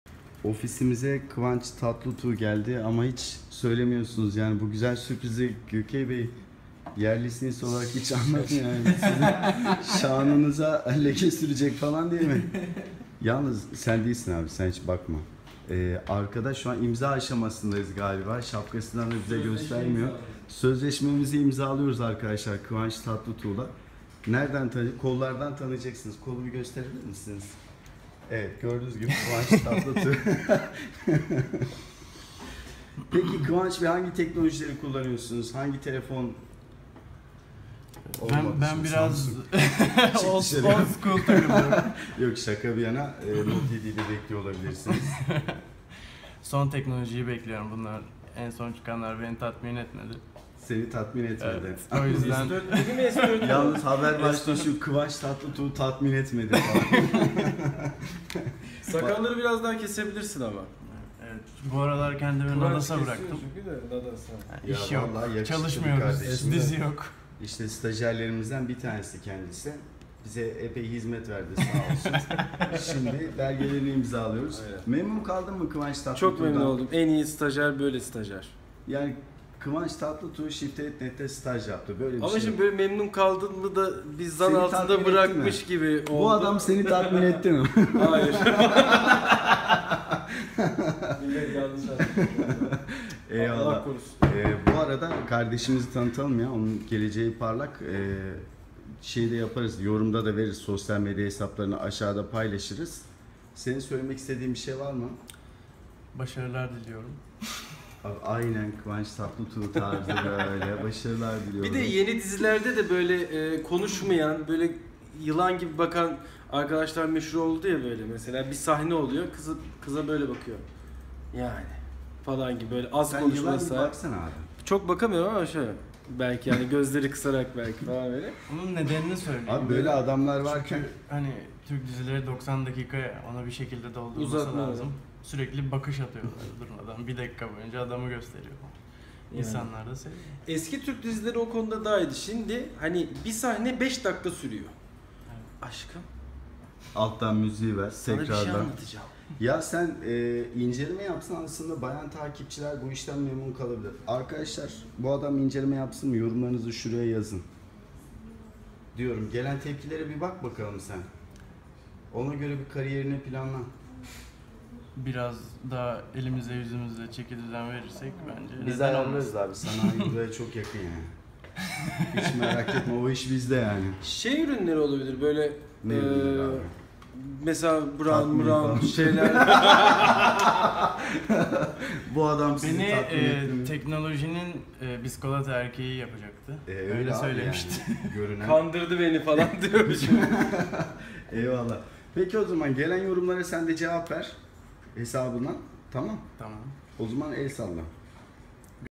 Kıvanç Tatlıtuğ'un Stajı Bitti 😅
SDN bünyesinde stajını tamamlayan ve Kıvanç Tatlıtuğ'a olan benzerliği ile ün yapan Mehmet'in stajı bitti :)

Bu videomuzda Mehmet'in kariyer planlarını konuştuk.